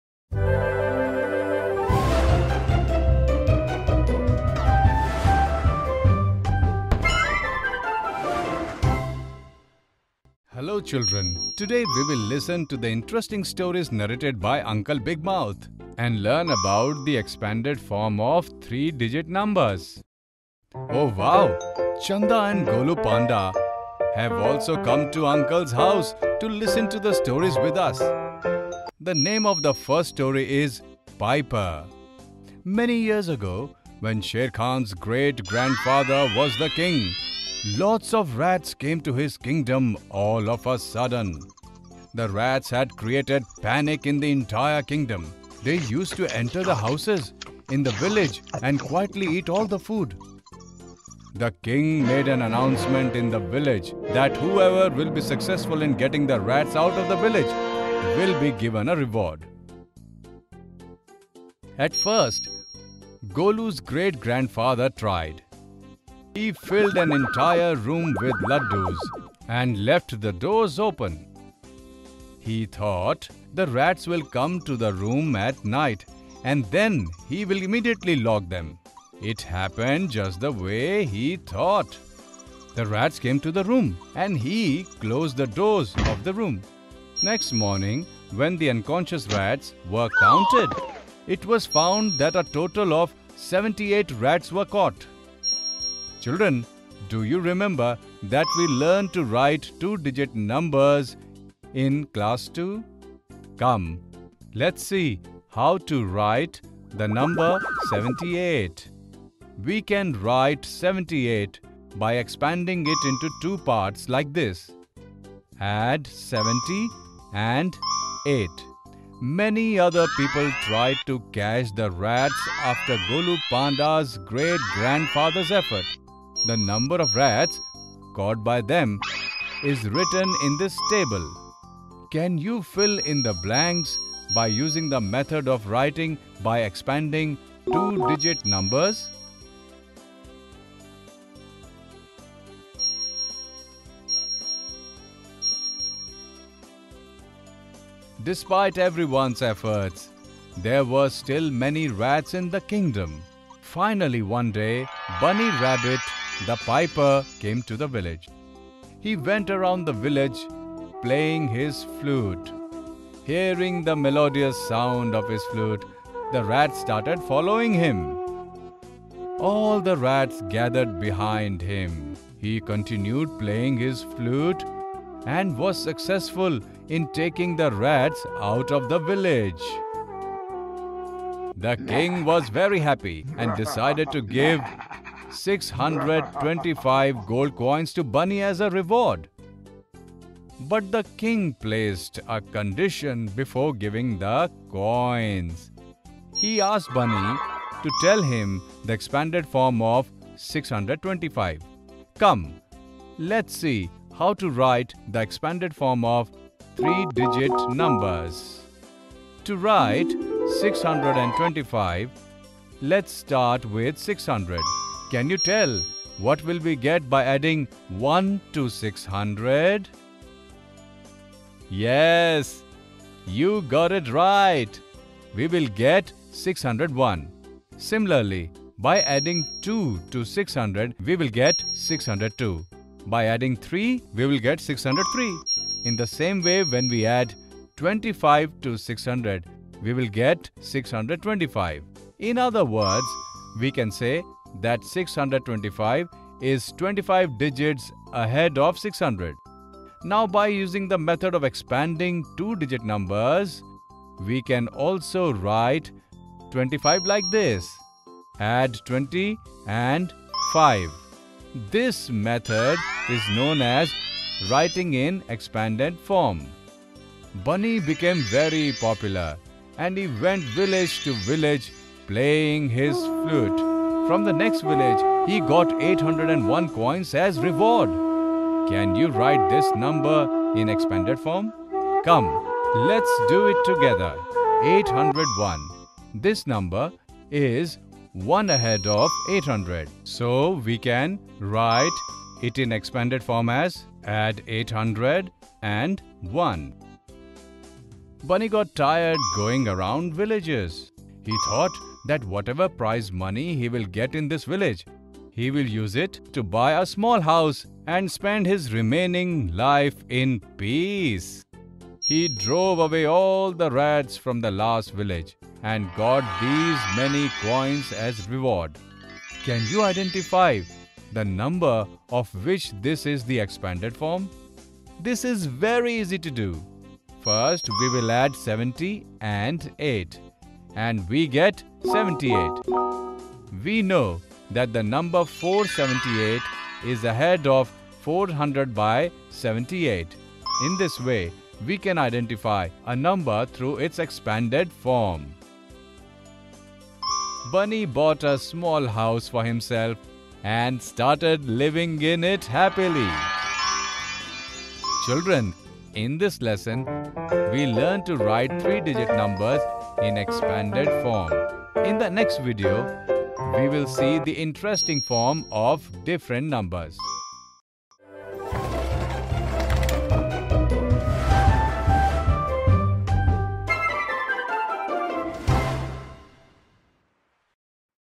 Expanded form of three digit numbers | Part 1/3 | English | Class 3
Expanded form of three digit numbers  | Part 1/3 | English | Class 3 |  | NCERT |

Expanded form of three digit numbers  | Numbers Upto 3 Digits | Grade 3 | NCERT | Fun with numbers

Welcome to part 1 of Expanded form of three digit numbers . In this video, we will discuss the concept of Expanded form of three digit numbers .